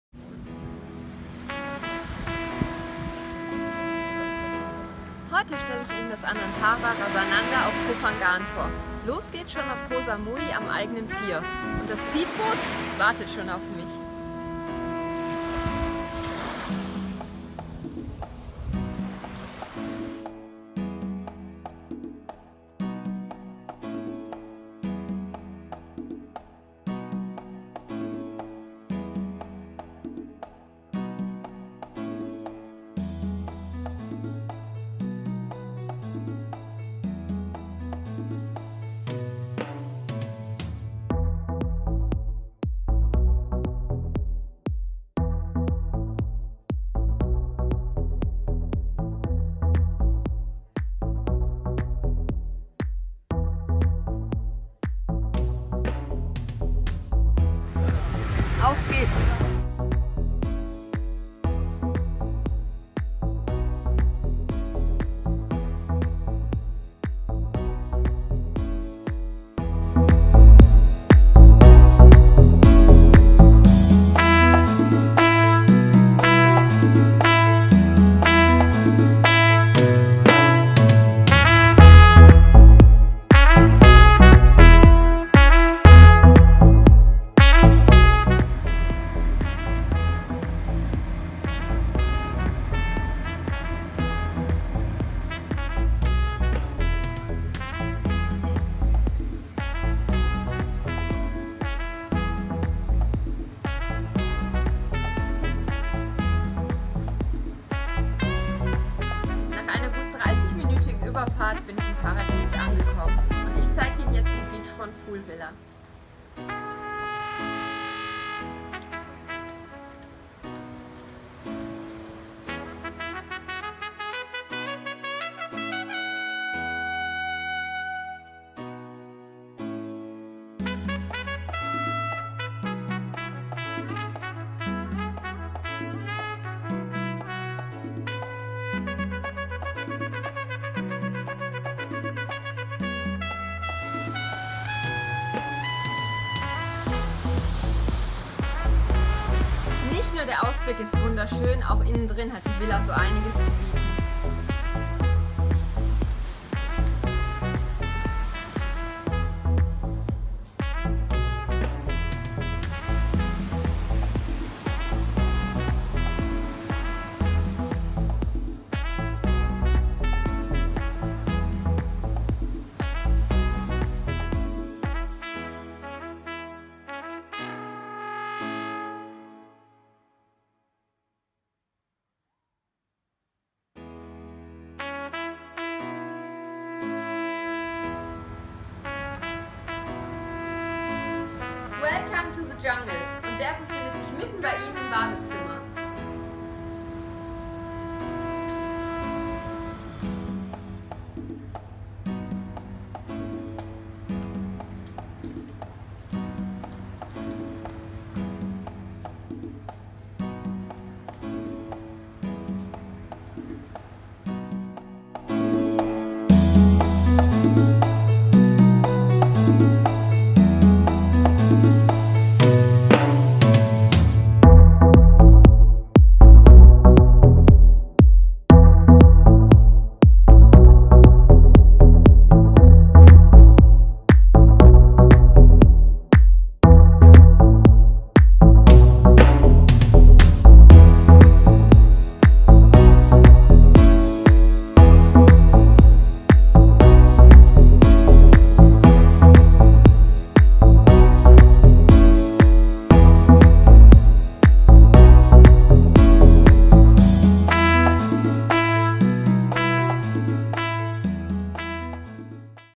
Anantara Rasananda Resort - Thailand 🇹🇭 Koh Phangan - Look Inside a Luxury Beachvilla
Das Anantara Resort liegt auf der thailändischen Trauminsel Koh Phangan direkt an einem traumhaften Sandstrand.

Ich nehme Sie mit auf eine Entdeckungstour.

Klicken Sie gerne mal rein!